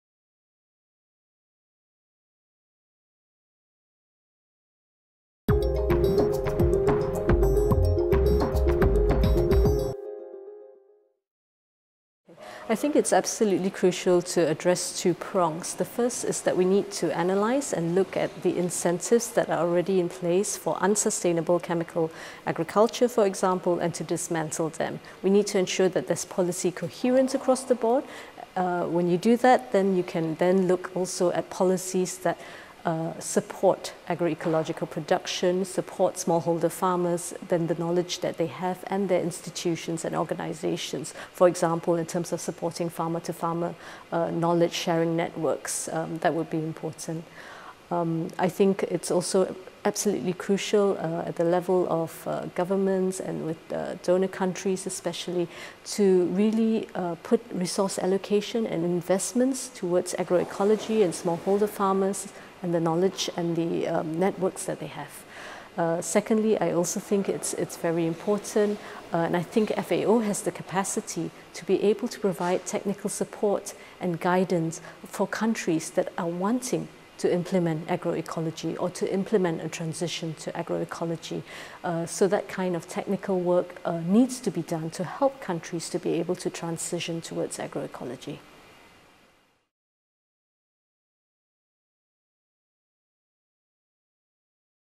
Lim Li Ching on establishing policies that promote agroecology
Lim Li Ching of the Third World Network underscores a two-prong approach to policies that create an enabling environment for the transition to agroecology.